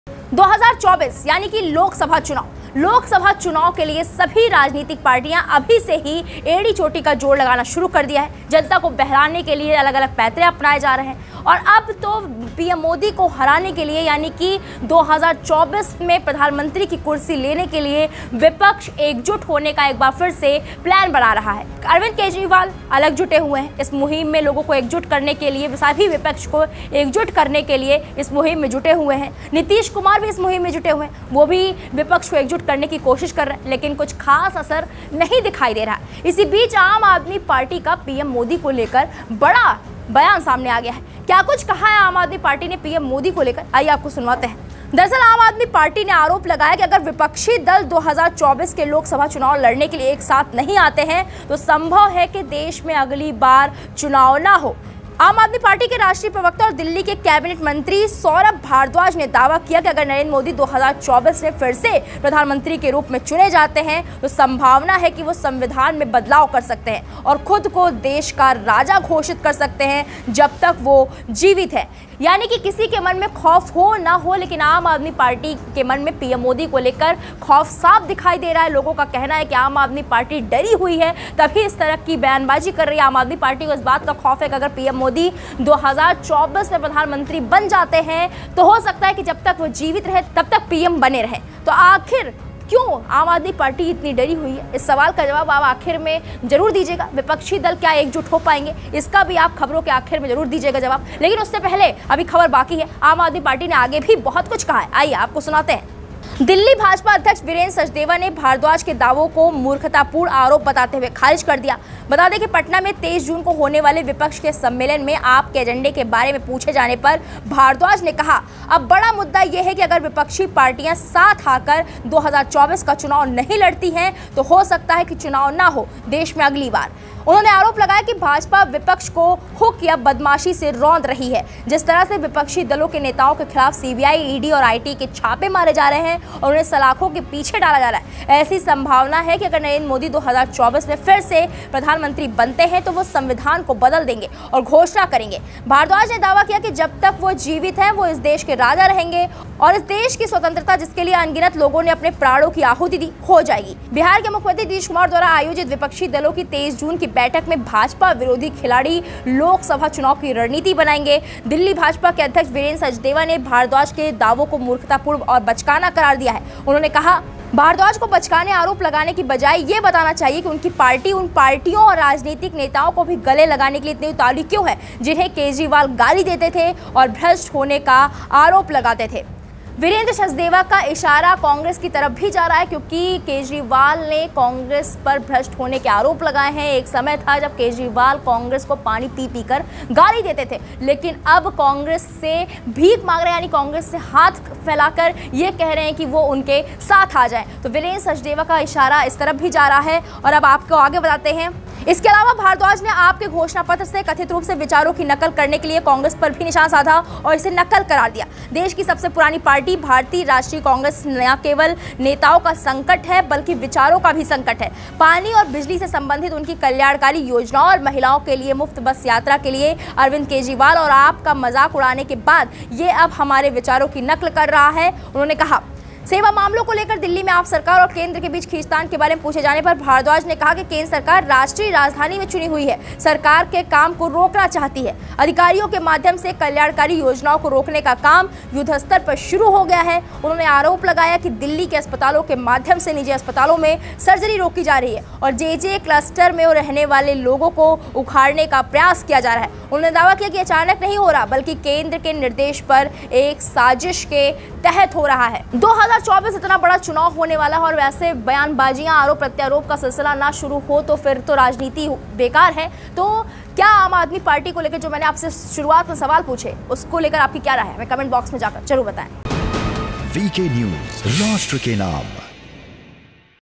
'...मरने तक पीएम रहेंगे मोदी, ये क्या बोल गयी आप? Election 2024 | Aap Party | PM Modi
एक प्रेस कॉन्फ्रेंस में बोलते हुए, AAP के राष्ट्रीय प्रवक्ता और दिल्ली के कैबिनेट मंत्री सौरभ भारद्वाज ने दावा किया कि अगर नरेंद्र मोदी 2024 में फिर से प्रधान मंत्री के रूप में चुने जाते हैं, तो संभावना है कि वह संविधान में बदलाव कर सकते हैं और खुद को देश का "राजा" घोषित कर सकते हैं। जब तक वह जीवित है।
Speaking at a press conference, AAP's national spokesperson and Delhi cabinet minister Saurabh Bhardwaj claimed that if Narendra Modi is re-elected as the prime minister in 2024, there are chances that he may make changes in the constitution and Can declare himself the "King" of the country. as long as he is alive.